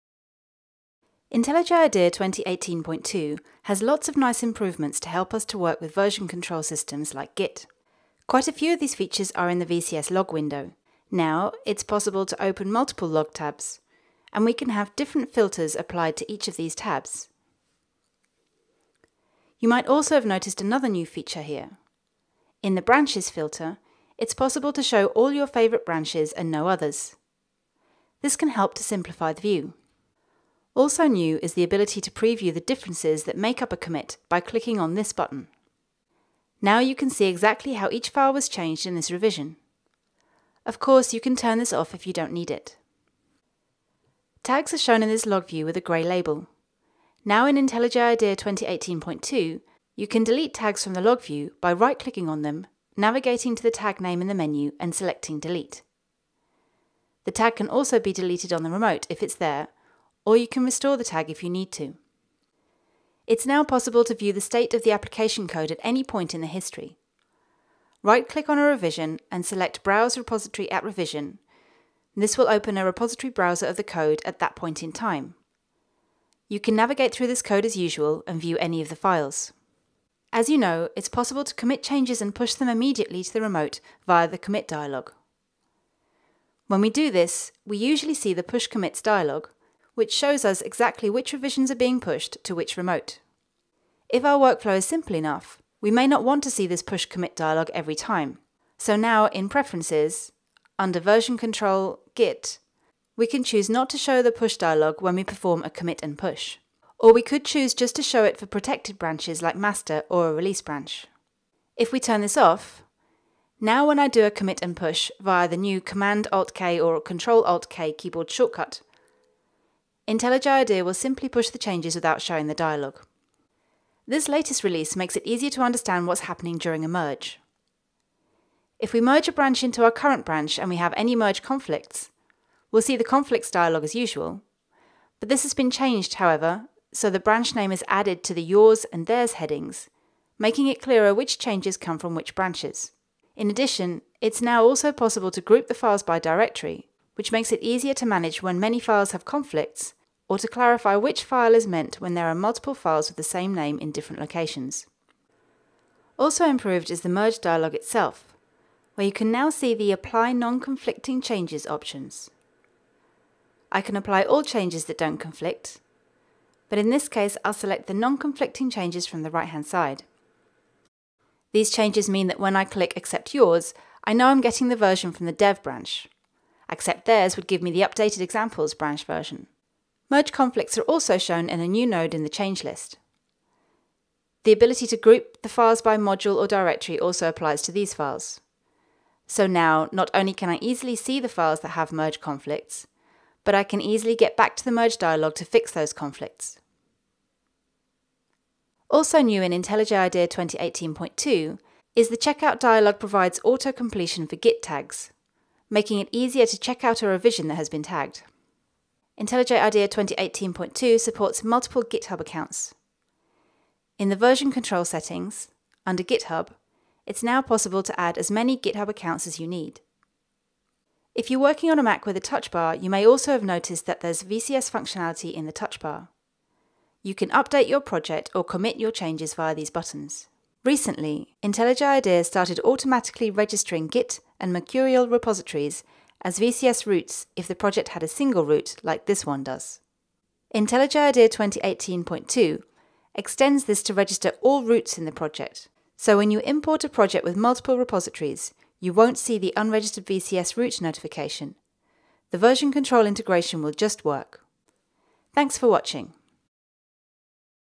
Version Control Systems Improvements in IntelliJ IDEA 2018.2
Improvements and additions for VCS support in IntelliJ IDEA 2018.2:
Log Window
 - You can open several Log tabs in the Version Control tool window.
 - The IDE now displays the Favorites branches in the Branch filter on the Log tab.
 - You can preview the diff in the VCS Log.
 - Tags can be deleted from Git commits via the context menu of the Log tab.
 - New Browse Repository at Revision action.

Committing
 - Skip the Push dialog completely when using Commit and Push, or display it only when committing to protected branches.

Merging
 - The updated Files Merged with Conflicts dialog displays Git branch names and adds a new Group files by directory option.
 - Merge conflicts node available for each changelist

Checkout
 - Autocompletion for Git tags is available in the Checkout dialog.

Other
 - Configure as many GitHub accounts as needed
 - Touch bar support
 - Automatically registers Git and Mercurial repositories as VCS roots